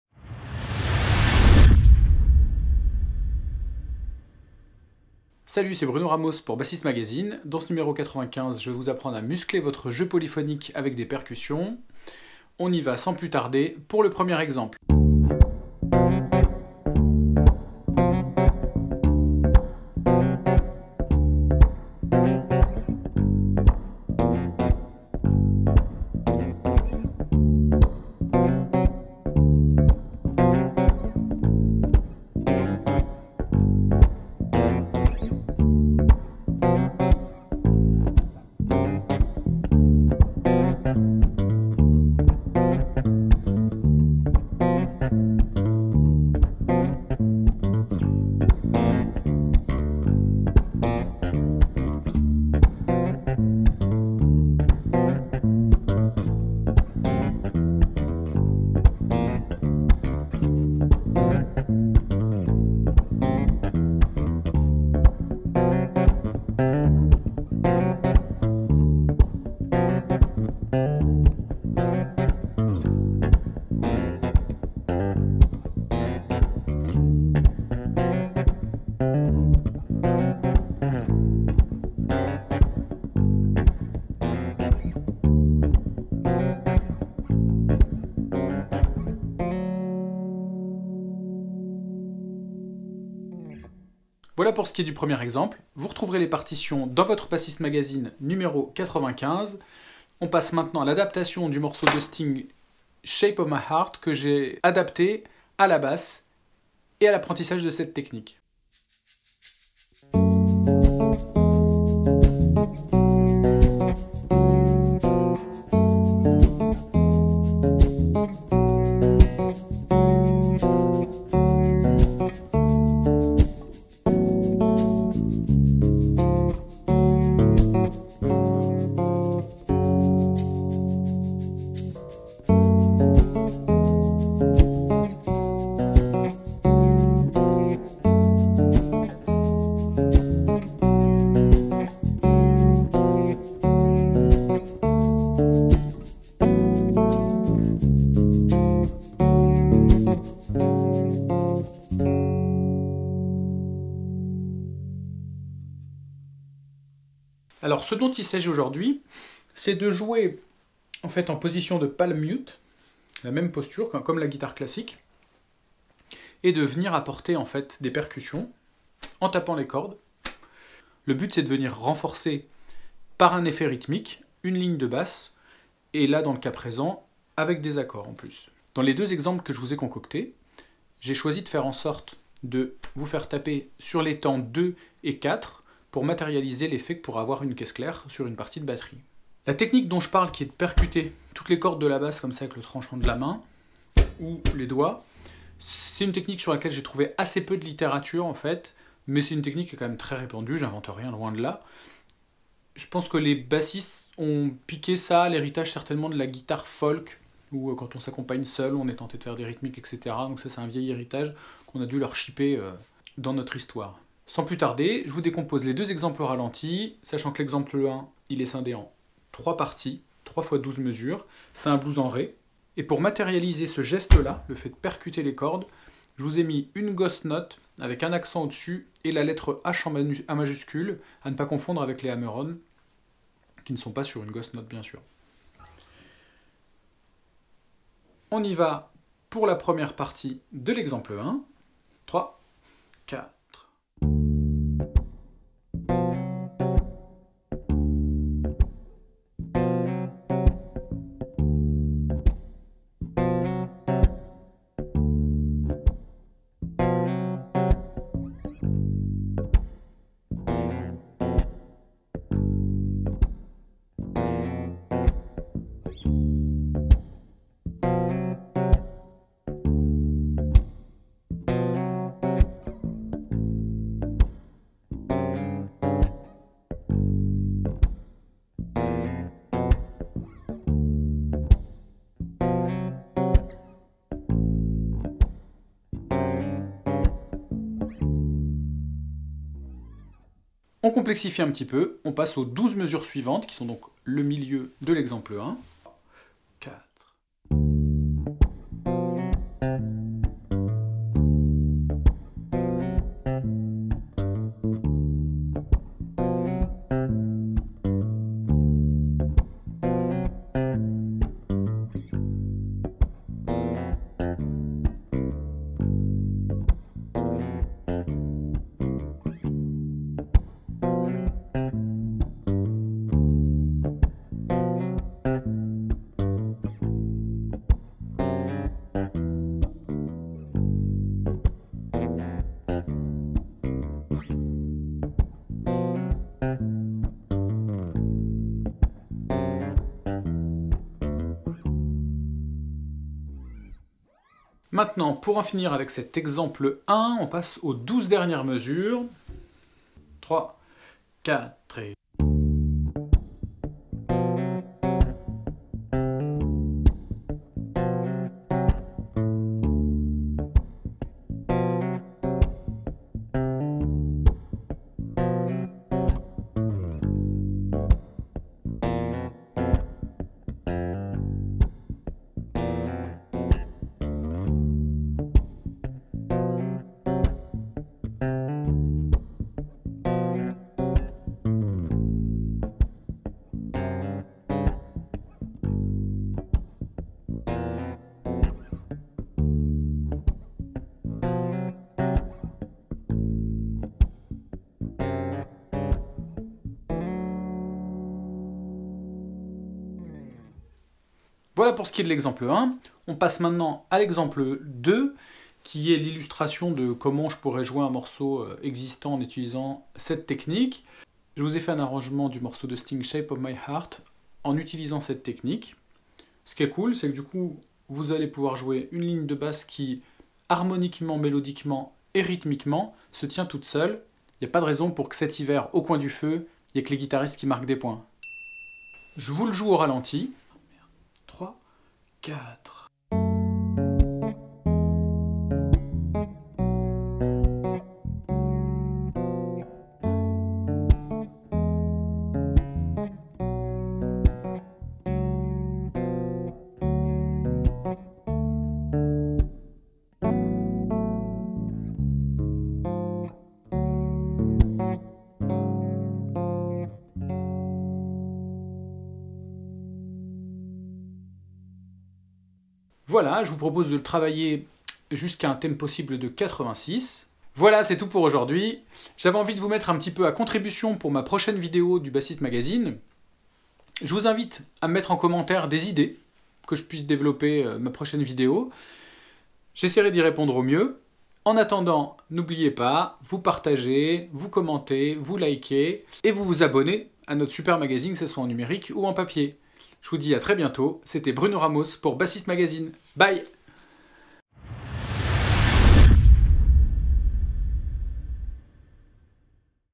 MUSCLEZ VOTRE JEU DE BASSE AVEC LES PERCUSSIONS - Bruno Ramos - Bassiste Magazine #95
Retrouvez les partitions de ce tuto dans le numéro 95 de Bassiste Magazine :